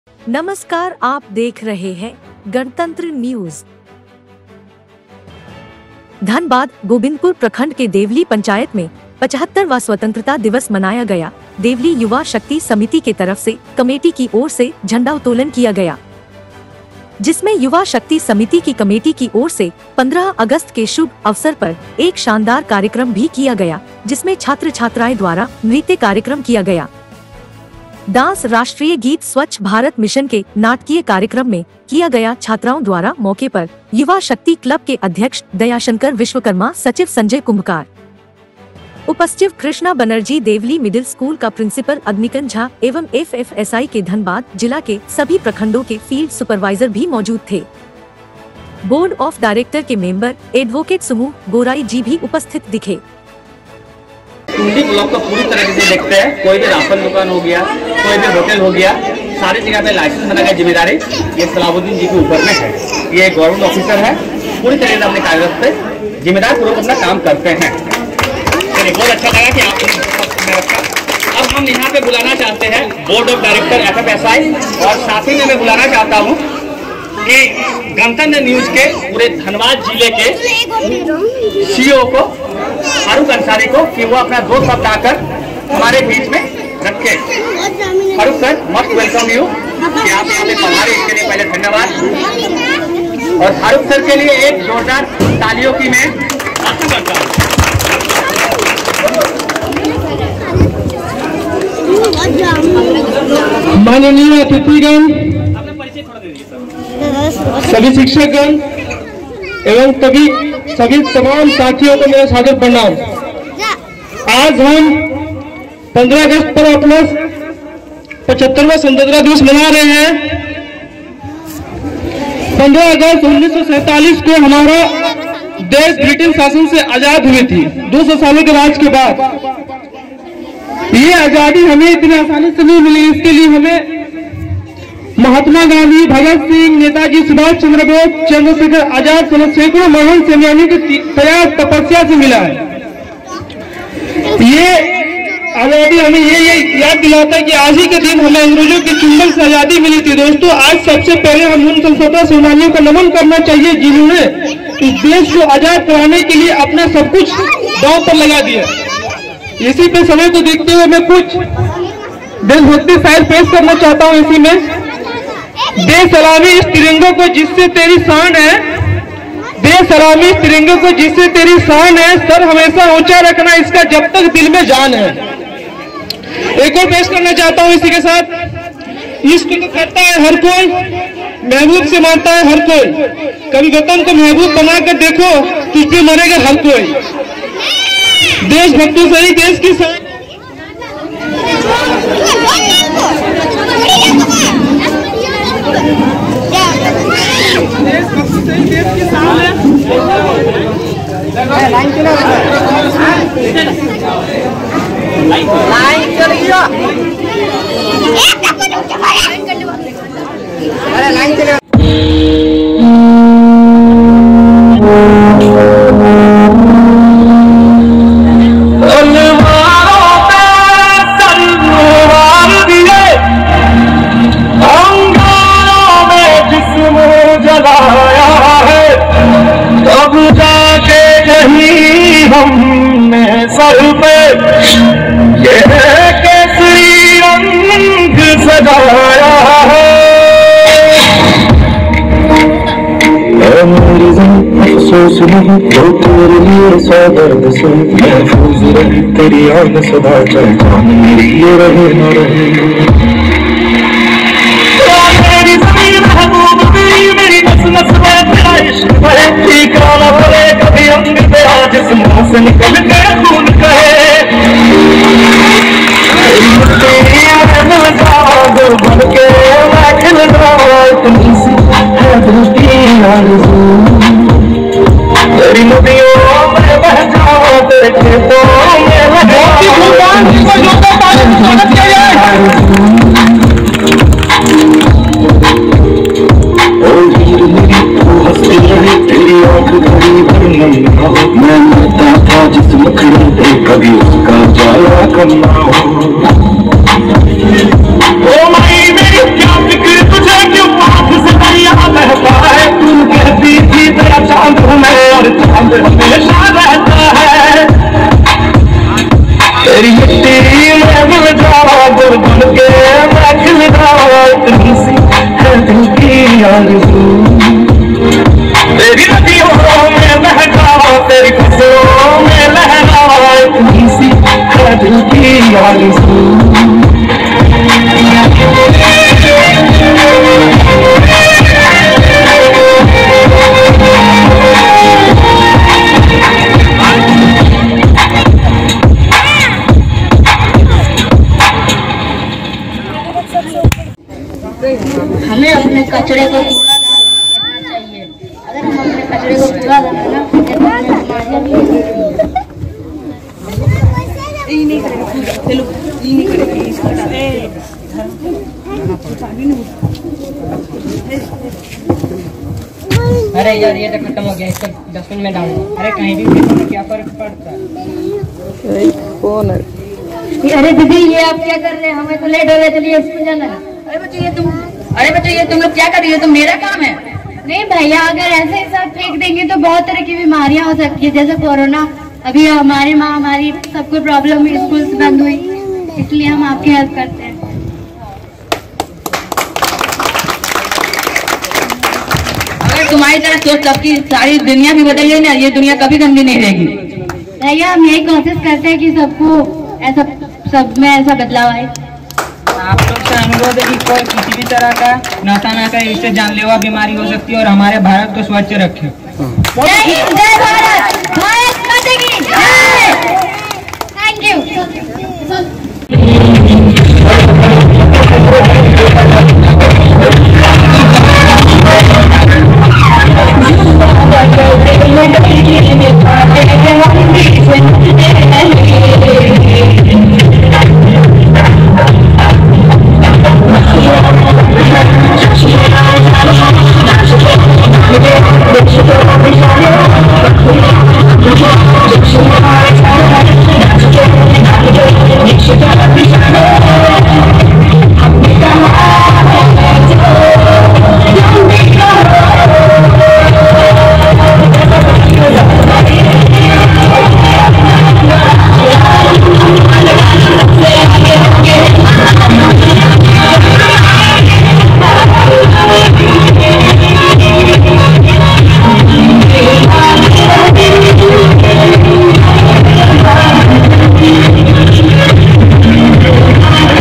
धनबाद गोबिन्दपुर प्रखंड के देवली पंचायत में 75 वा स्वतंत्रता दिवस मनाया गया देवली युवा शक्ति समिति
धनबाद- गोबिन्दपुर प्रखंड के देवली पंचायत में 75 वा स्वतंत्रता दिवस मनाया गया देवली युवा शक्ति समिति के तरफ से कमेटी की ओर से झंडाउतोलन किया गया जिसमें युवा शक्ति समिति की कमेटी की ओर से 15 अगस्त के शुभ अवसर पर एक शानदार कार्यक्रम भी किया गया जिसमें छात्र-छात्राएं द्वारा नृत्य कार्यक्रम किया गया डांस राष्ट्रीय गीत स्वच्छ भारत मिशन के नाटकीय कार्यक्रम में किया गया छात्राओं द्वारा मौके पर युवा शक्ति क्लब के अध्यक्ष दयाशंकर विश्वकर्मा सचिव संजय कुंभकार उपसचिव कृष्णा बनर्जी देवली मिडिल स्कूल का प्रिंसिपल अग्निकन झा एवं एफएफऐसाई के धनबाद जिला के सभी प्रखंडों के फील्ड सुपरवाइजर भी मौजूद थे बोर्ड ऑफ डायरेक्टर के मेंबर एडवोकेट सुमूह गोराई जी भी उपस्थित दिखे

Ganatantra News - provides latest news from India and World on Politics,Technology,Entertainment,Sports etc.